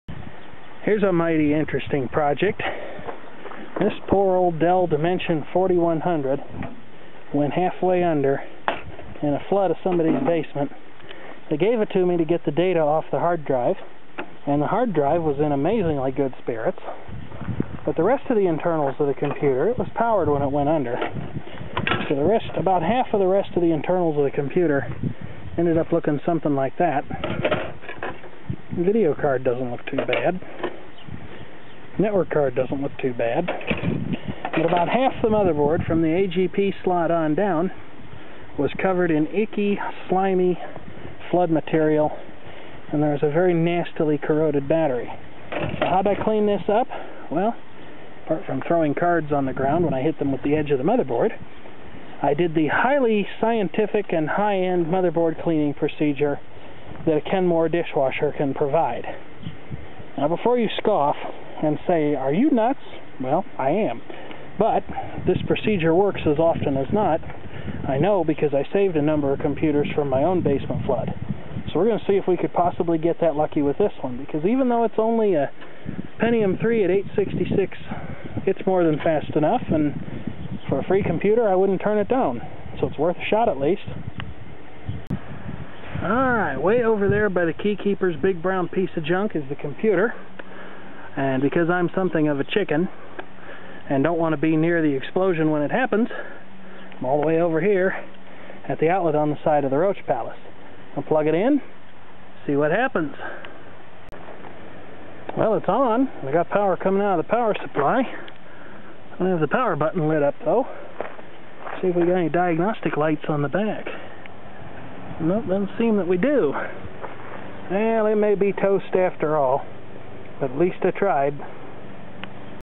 Reviving a Flooded Computer
What can I say? It was a free PC, which is always good. And you can still do a lot with an older computer!

I noticed that the i815 Northbridge was getting very hot, so my guess is that it failed or something attached directly to it was bad. And I never saw any rear diagnostic lights come on, nor did I ever hear any POST beeps.